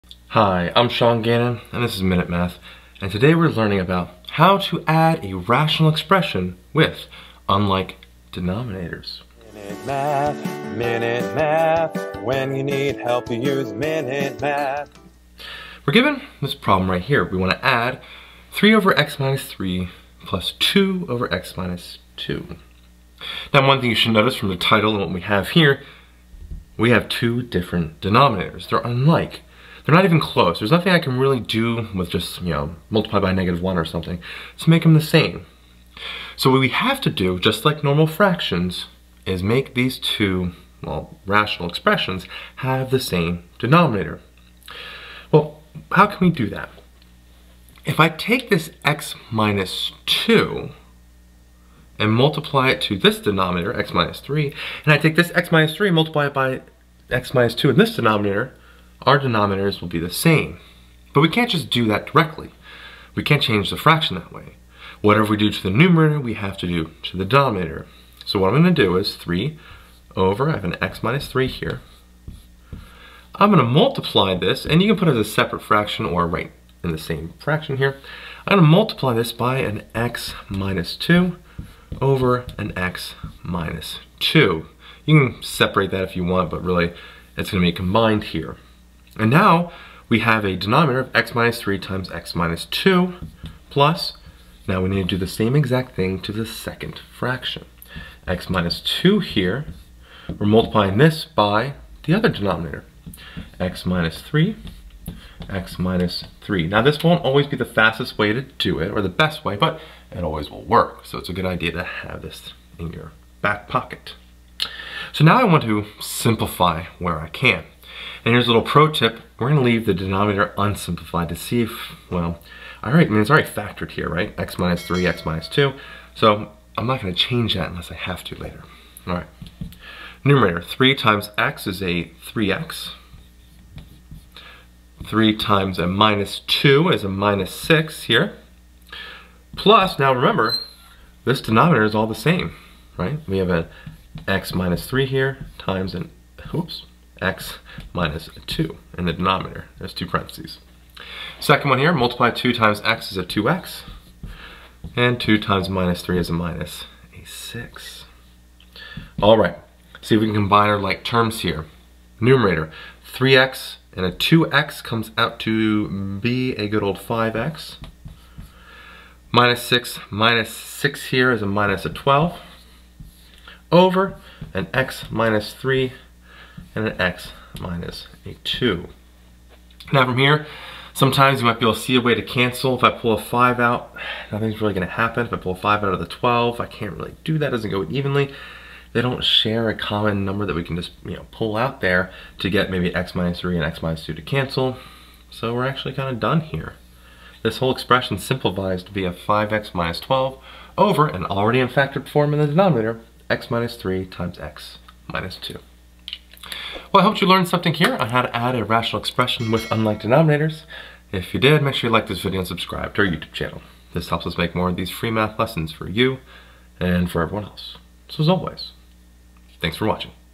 How to Add a Rational Expression with Unlike Denominators | Minute Math
In this video we learn How to Add a Rational Expression with Unlike Denominators.

Marecek, L., & Mathis, A. H. (2020). Add and Subtract Rational Expressions. In Intermediate Algebra 2e. OpenStax.